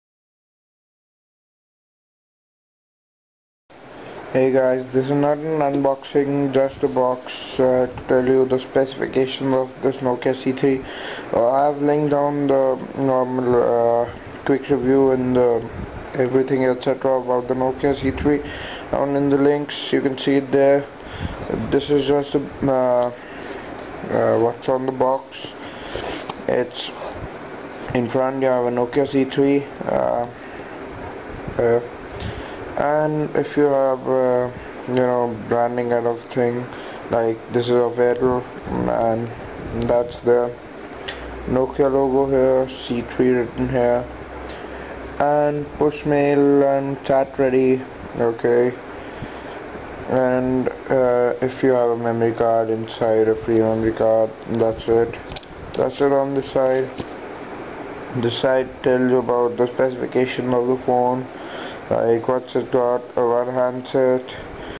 Nokia C3 Quick Review
Beetel Bite gets hands on the Nokia C3-00 and does a quick review.
leave any sujestions or questions in our twitter
or
fb you surely will be answred.
or you could just comment